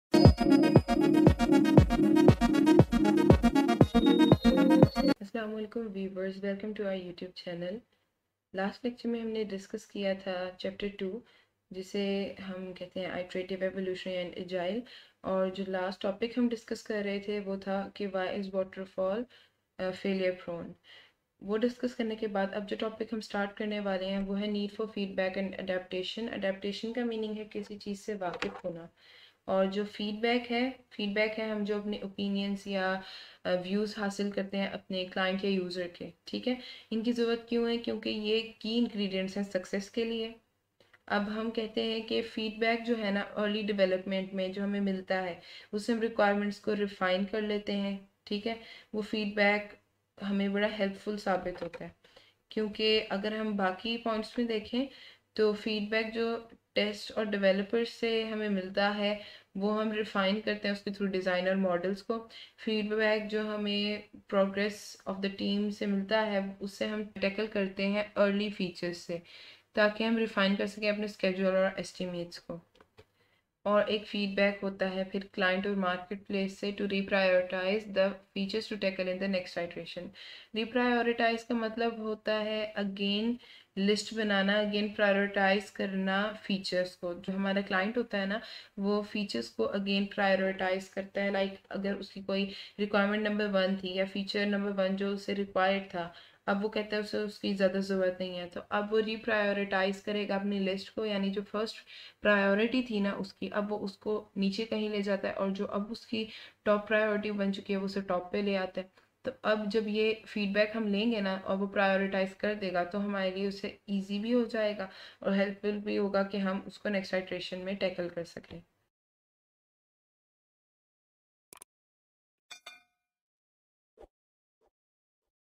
Why there is need for feedback and adaptation Complete Urdu Tutorial
This Urdu Tutorial shows the basic need  of taking feedback. An Adaptive Feedback System to Improve Student Performance Based on Collaborative Behavior.
The success of these systems relies on engaging experience and timely and accurate feedback to the students on their performance.

All necessary discussion is available in this Urdu Lecture...!

Learn to Achieve Your Goals….!
Do Like 👍
Do Comment ✌
Do Share 📢